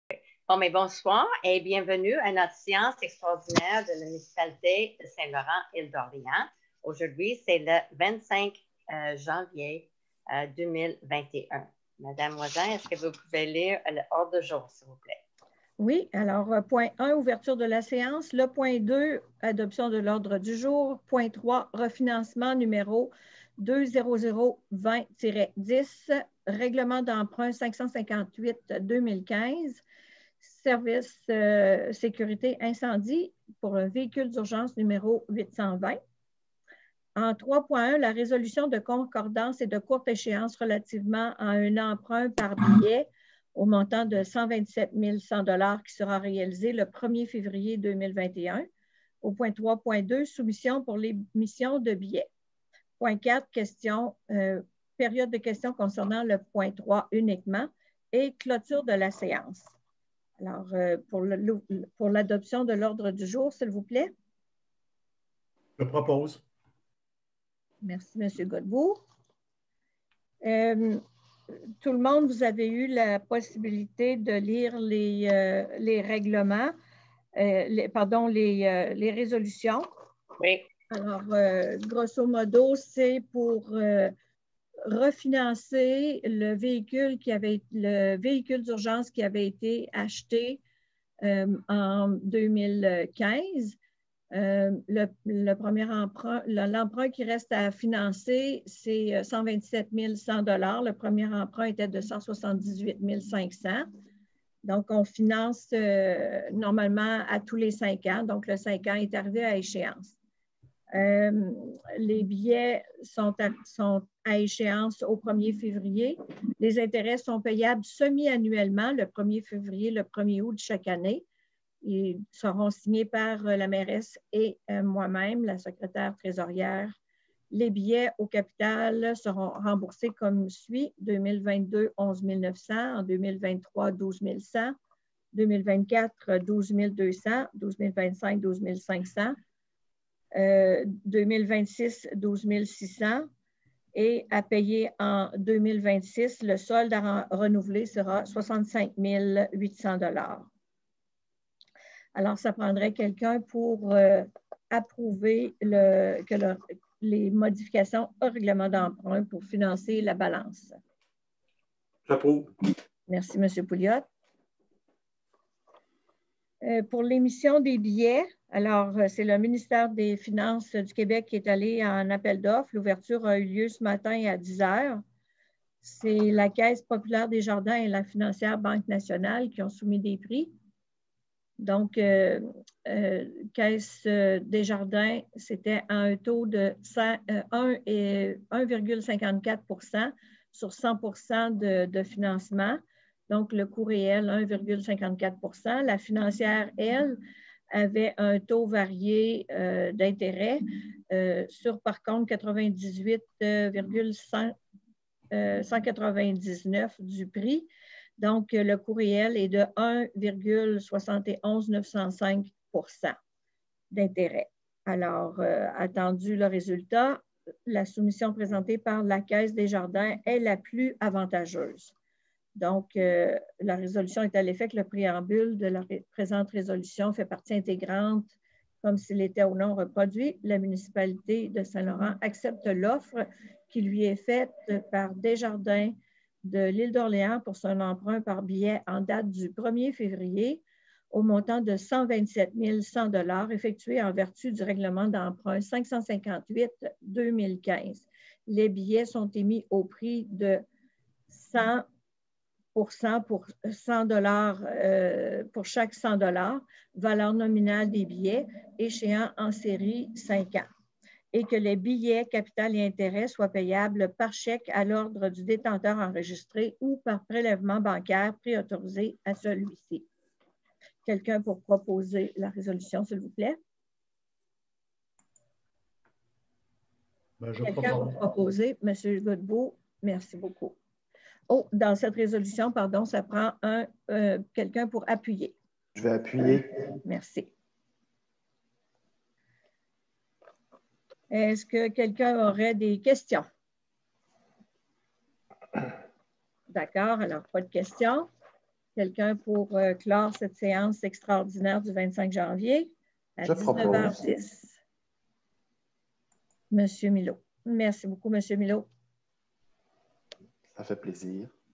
Municipalité de Saint-Laurent - Séance 2021-01-25
Séance du conseil municipal de la municipalité de Saint-Laurent-de-l'île-d'Orléans tenue le 25 janvier 2021.